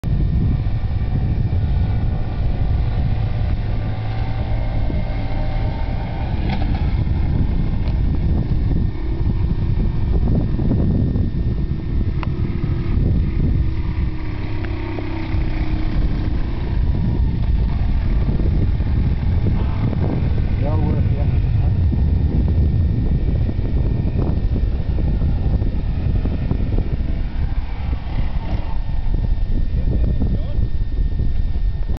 Honda C50 Cubra on Stwlan Drive
Slow hill climb for the standard Honda C50 Cubra